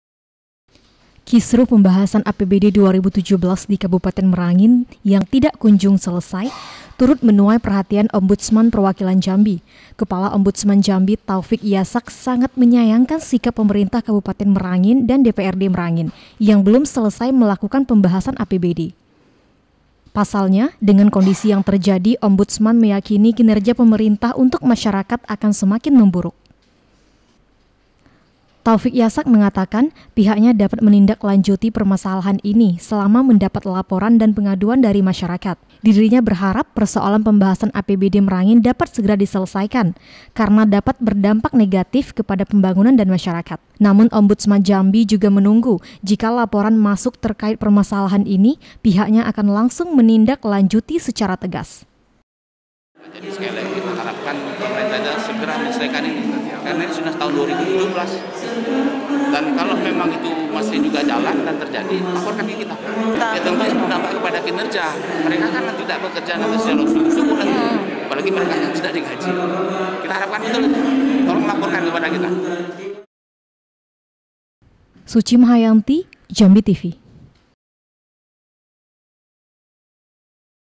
OMBUDSMAN SAYANGKAN KISRUH PEMKAB DAN DPRD MERANGIN
KISRUH APBD MERANGIN TURUT MENJADI PERHATIAN OMBUDSMAN JAMBI, KELAKUAN PEMKAB DAN DPRD MERANGIN SANGAT DISAYANGKAN SEMUA PIHAK.